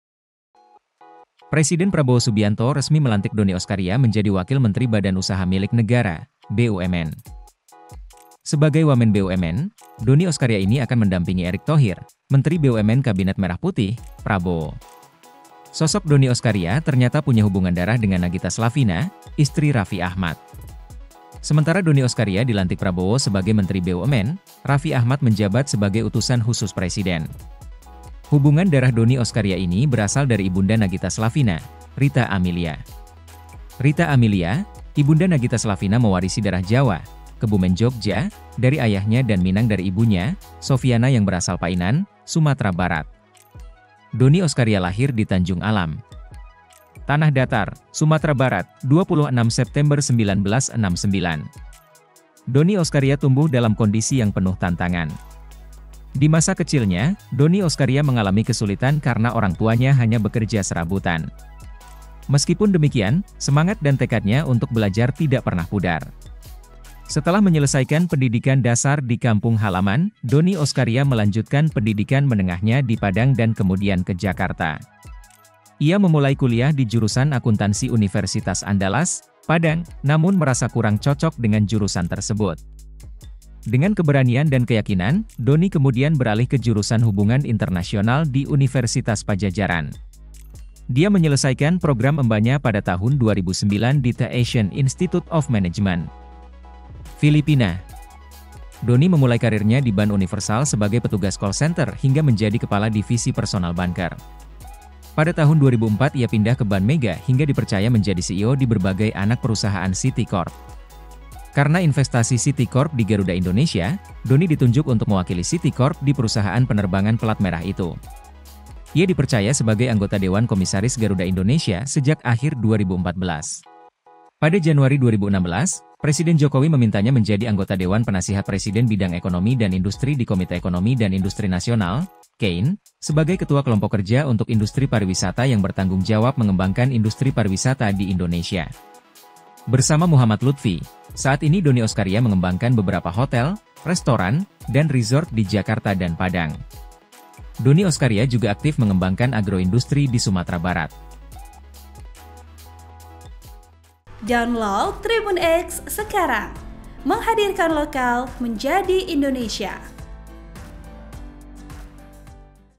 Profil Dony Oskaria, Wamen BUMN, Ada Hubungan Darah dengan Istri Raffi Ahmad, Paman Nagita Slavina
Presiden Prabowo Subianto resmi melantik Dony Oskaria menjadi Wakil Menteri Badan Usaha Milik Negara (BUMN).
Sebagai Wamen BUMN, Dony Oskaria ini akan mendampingi Erick Thohir, Menteri BUMN Kabinet Merah Putih, Prabowo.
Sosok Dony Oskaria ternyata punya hubungan darah dengan Nagita Slavina, istri Raffi Ahmad.
Sementara Dony Oskaria dilantik Prabowo sebagai Menteri BUMN, Raffi Ahmad menjabat sebagai Utusan Khusus Presiden.
Hubungan darah Dony Oskaria ini berasal dari ibunda Nagita Slavina, Rieta Amilia.
Rieta Amilia, ibunda Nagita Slavina mewarisi darah Jawa (Kebumen Jogja) dari ayahnya dan Minang dari ibunya, Sofiana yang berasal Painan, Sumatera Barat.
Dony Oskaria lahir di Tanjung Alam, Tanah Datar, Sumatera Barat, 26 September 1969.

Editor: Jofan Giantirta